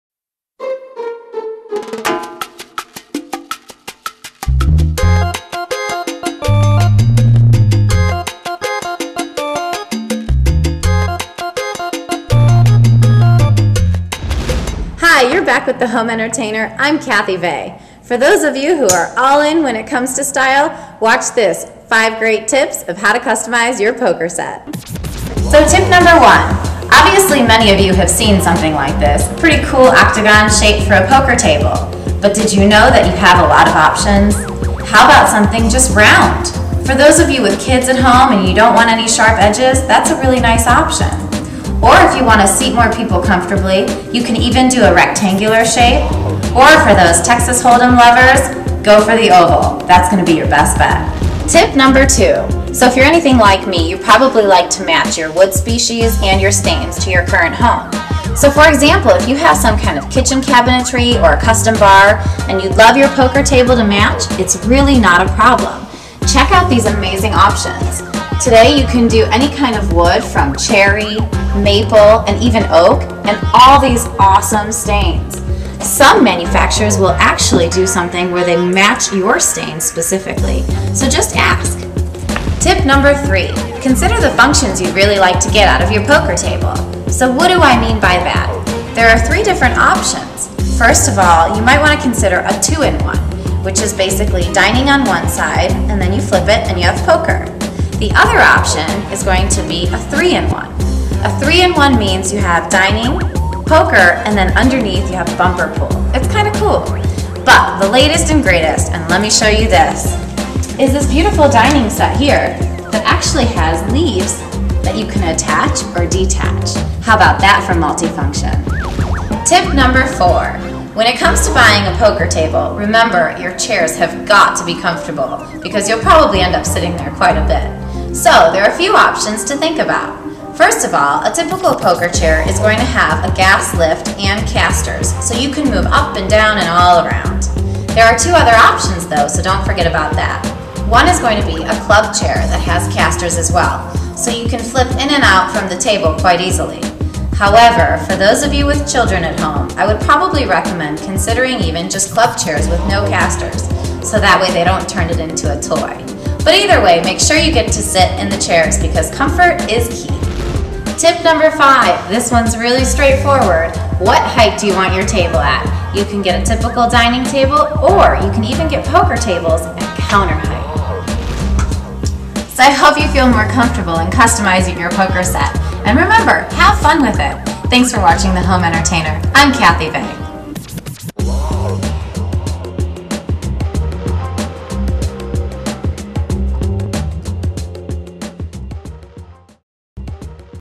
POKER TABLE | CUSTOM OPTIONS
Kathy Vegh, The Home Entertainer discusses 5 ways you can customize your poker set. In this short segment she covers the key questions you need to ask yourself when considering a purchase 1) What shape should your table be? 2) What are your options when it comes to wood type and wood finishes? 3) What kind of functionality do you want from the table?  Dining and Poker?  Or Dining, Poker and Bumper Pool? 4) What should you consider when picking your chairs? 5) What height you would like your poker table to be?  All exciting options that allow you to make your poker table unique to you and your needs.